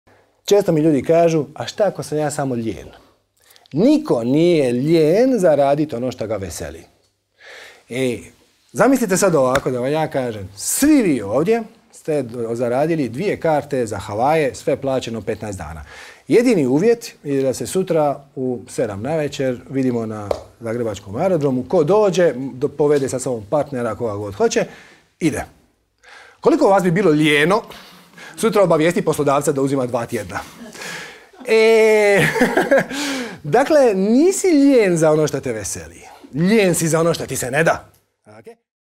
Srđan Roje: "A što ako sam ja lijen?"
Kratak i jednostavan odgovor na često postavljano pitanje: "a kako slijediti svoju strast ako sam ja jednostavno lijen"?

Izvadak iz predavanja "Pravi zakon privlačenja" kojeg u cijelosti možete pogledati na